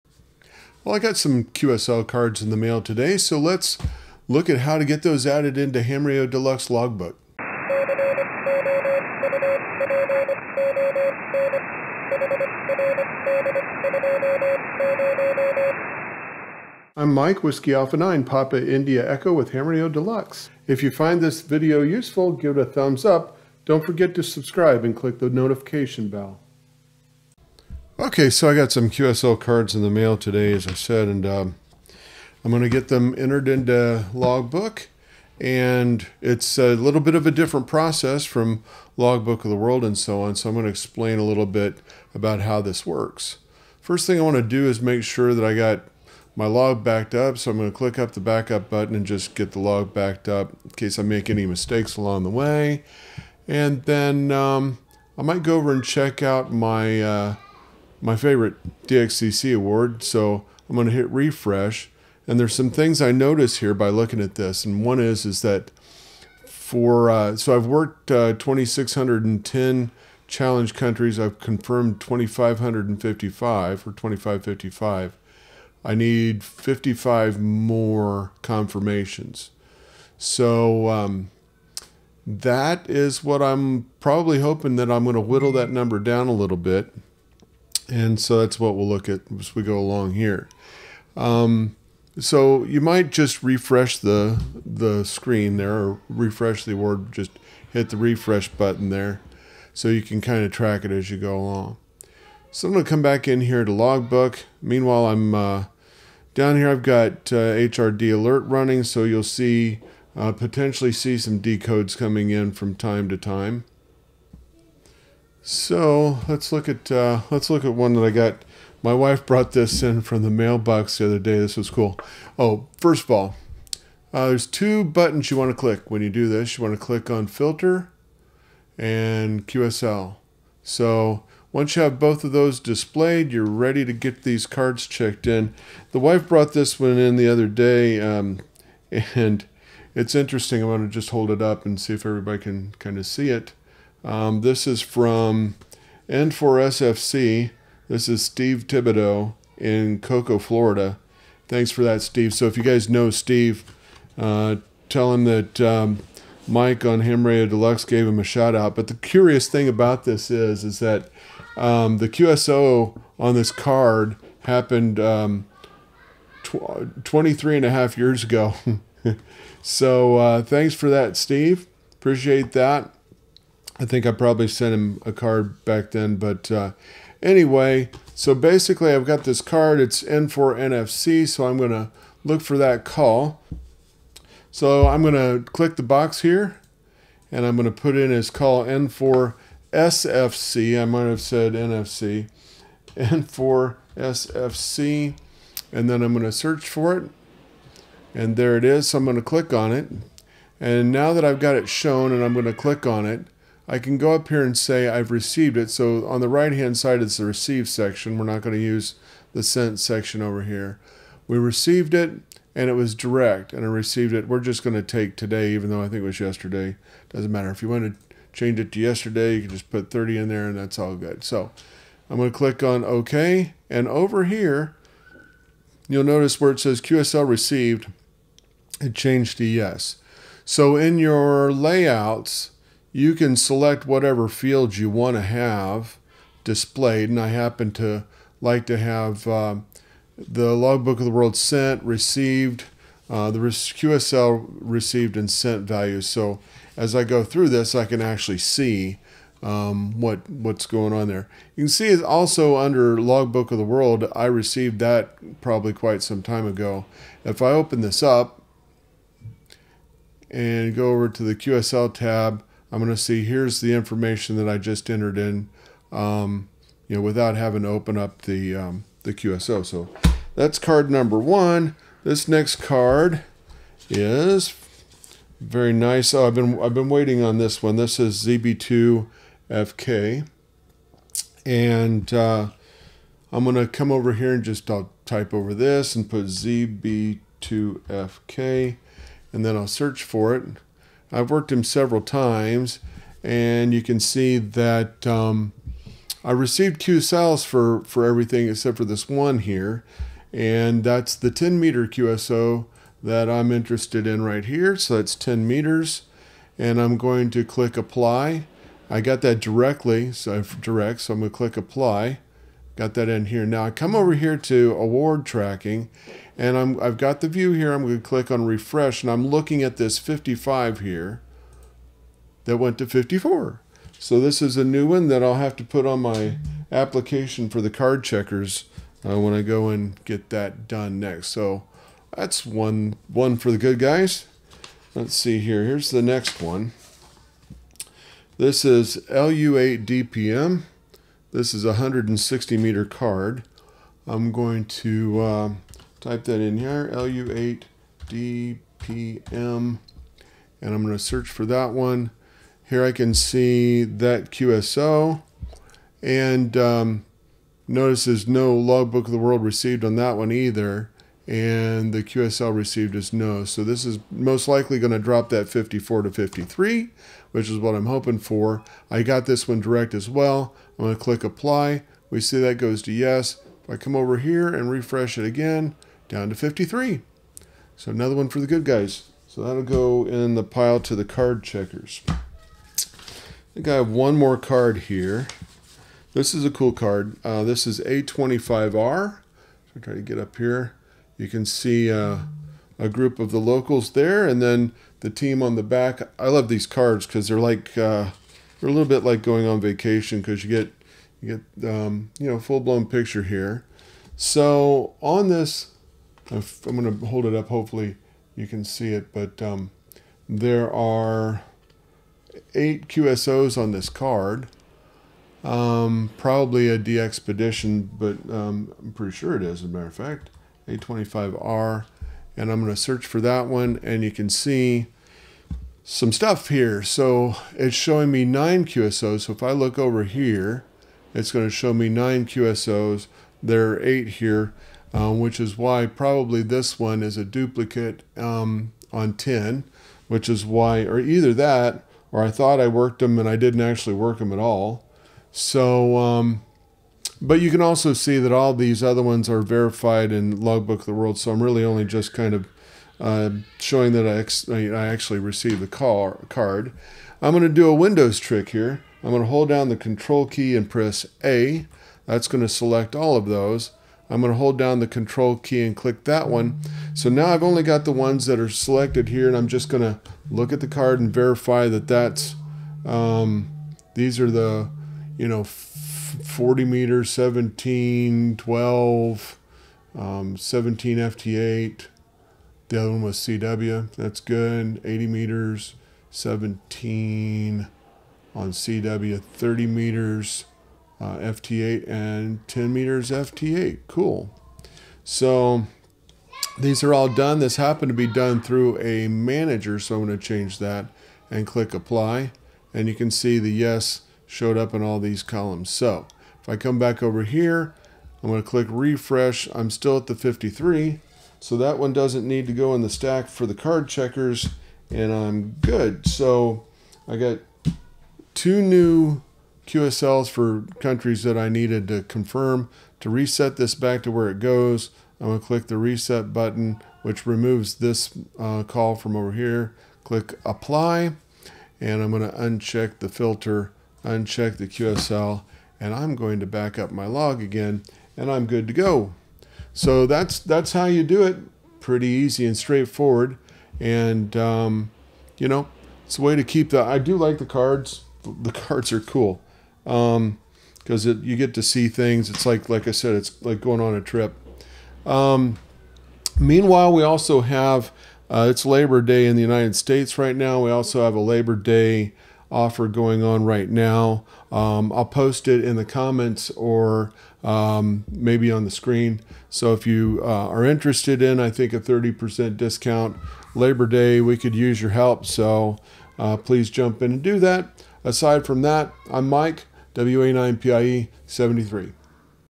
Checking QSL Cards into Ham Radio Deluxe® Logbook
In this video, I'm going to demonstrate the process for checking in QSL cards in Ham Radio Deluxe® Logbook.

Offer ends at midnight on 1-September 2024.
Good for the purchase for a new customer or a renewal for an existing customer.